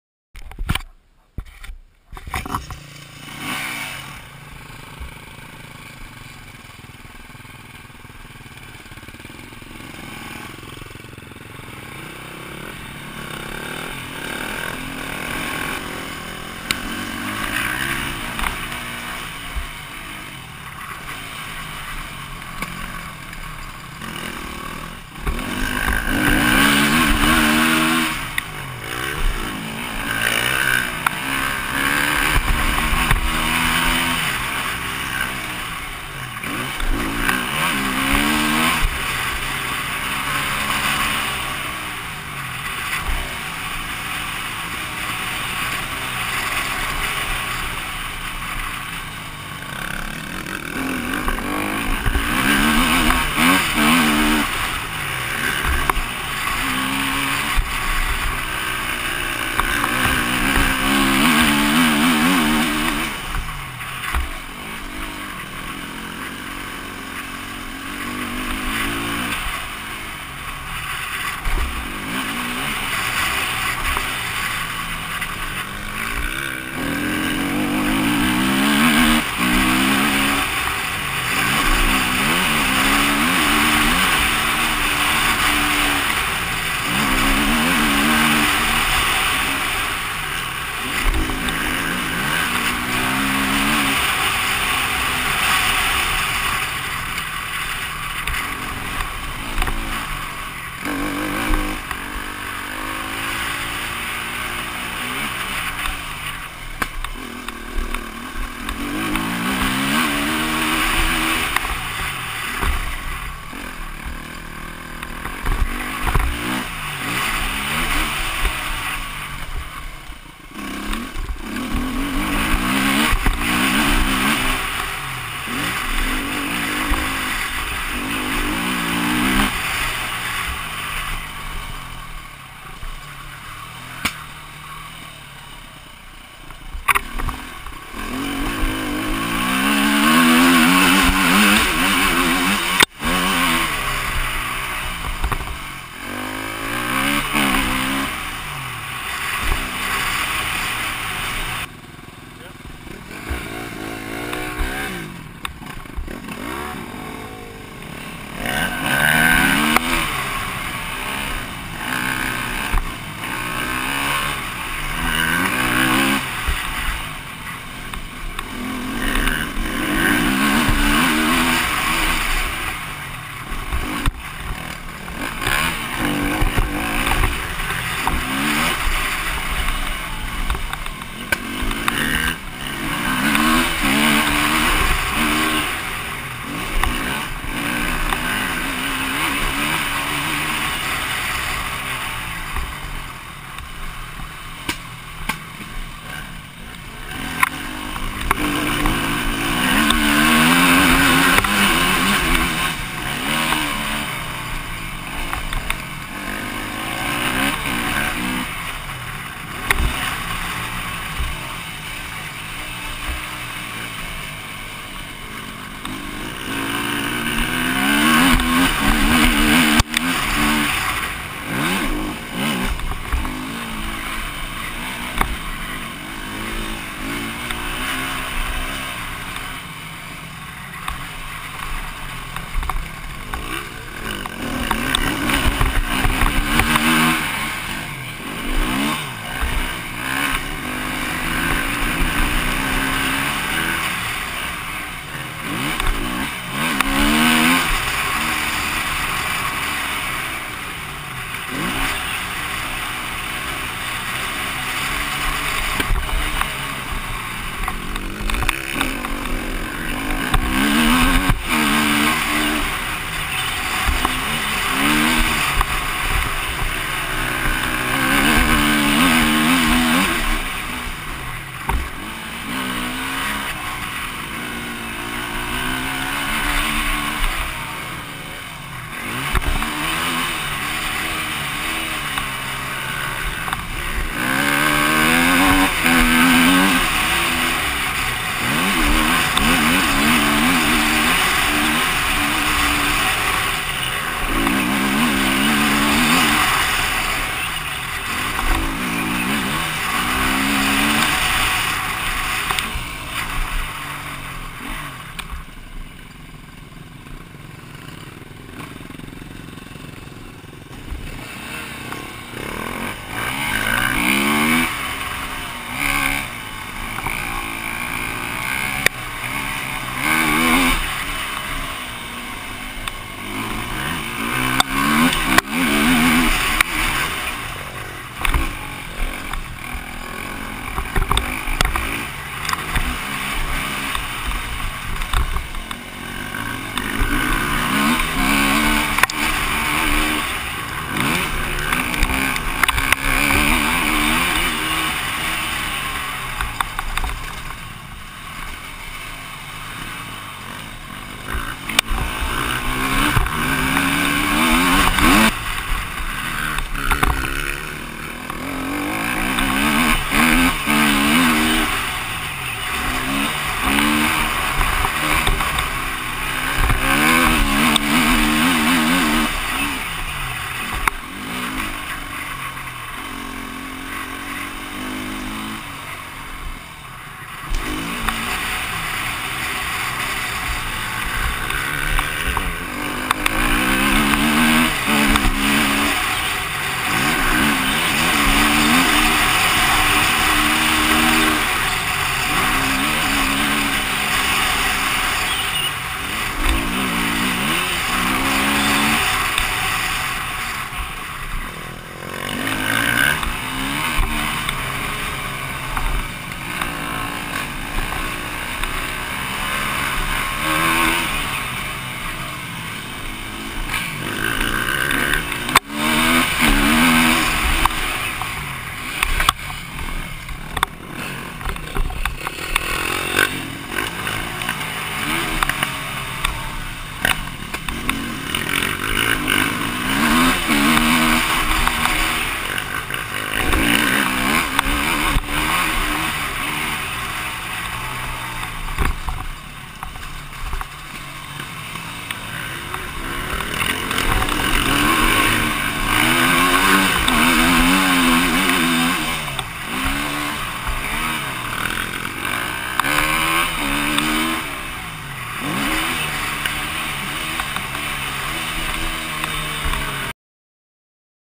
KTM EXC-f 250 // Nagycenk Motocross 2013 // GoPro [HD Hero]
Awesome riding day on a perfect track in Nagycenk (Hungary).